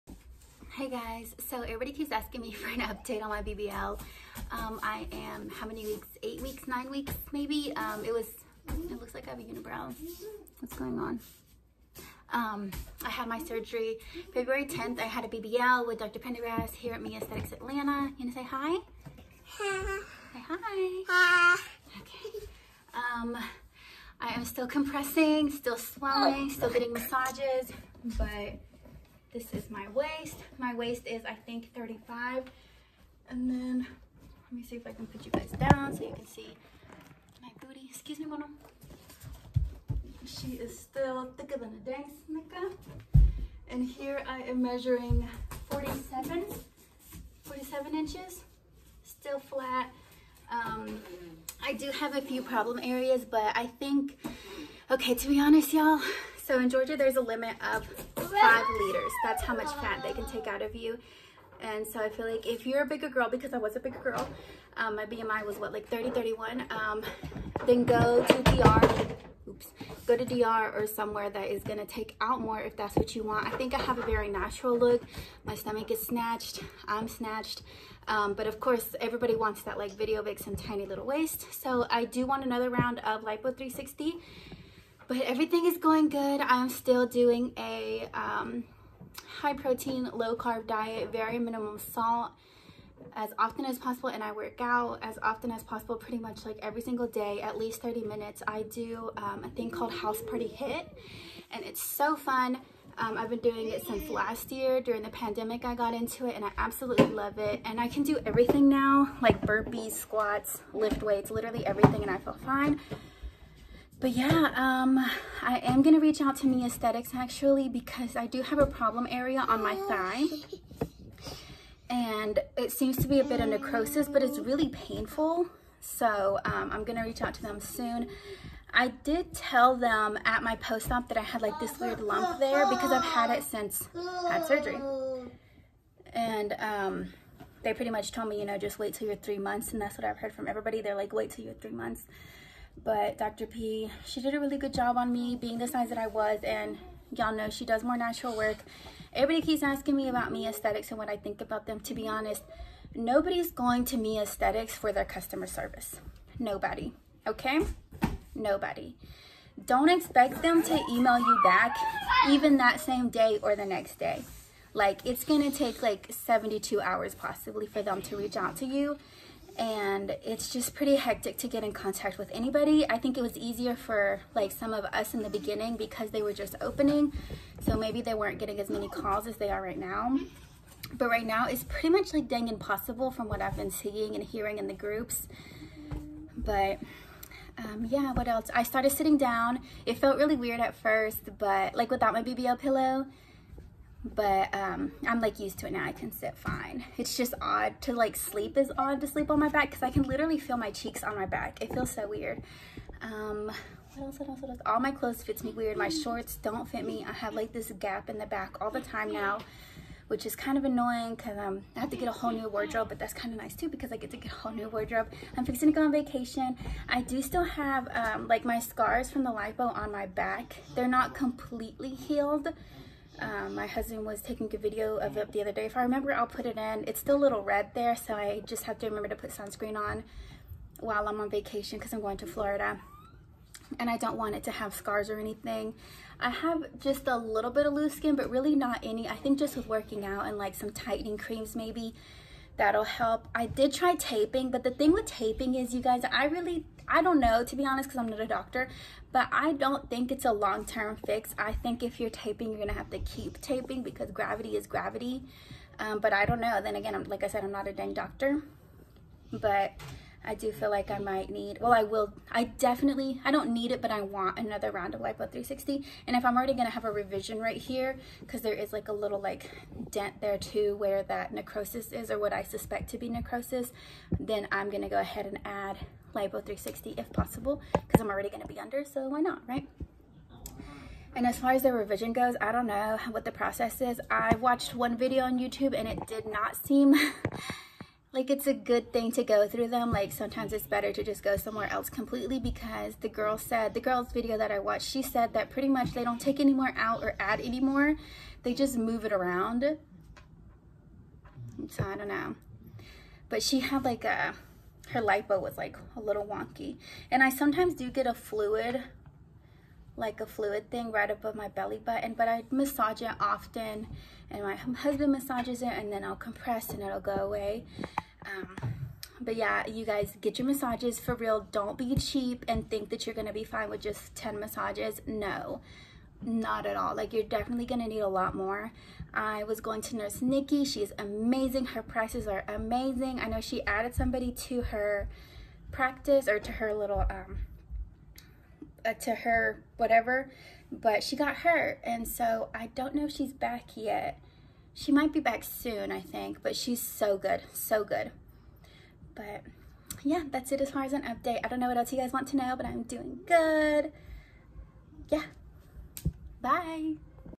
Update on Bbl 2 month post op| Dr Prendergast | Mia Aesthetics Atl
Hey guys here is a quick update on my bbl. sorry I haven’t posted in a while. Also Mia Aesthetics Atlantas Instagram posted me abs my results if you guys want to check it out I am the one in the black dress 😌. Dr p does more natural work & keep in mind most round ones if you are a bigger girl have more natural results since there is a limit on how much fat you can get taken out. No I did not need a tummy tuck lolol and to those saying I wasted my money why are you pressed about MY money & MY results??? I am happy with my results minus the one little area and I think Dr P gave me a great base for a round 2 🤪 y’all know I wanted more hips!!